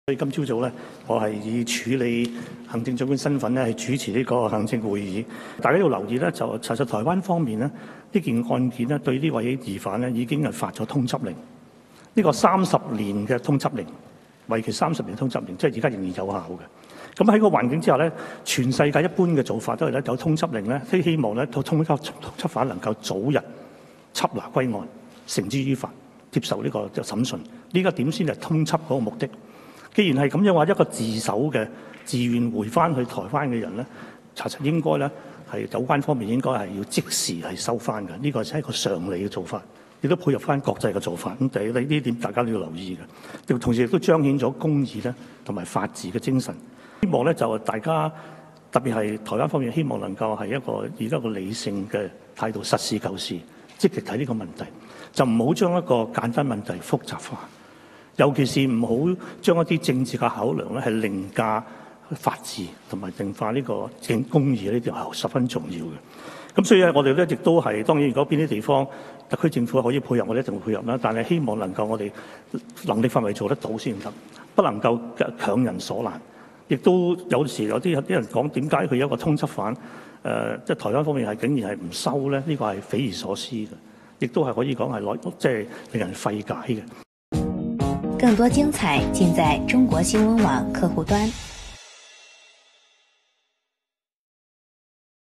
台湾当局拒绝接收陈同佳 香港特区政府指其匪夷所思
10月22日上午，香港特区署理行政长官张建宗出席行政会议前会见传媒时，回应陈同佳自愿自首案件。对于台湾当局拒绝接收陈同佳，香港特区政府感到匪夷所思。他希望台湾以务实的态度，实事求是，不要政治凌驾法治，勿强人所难。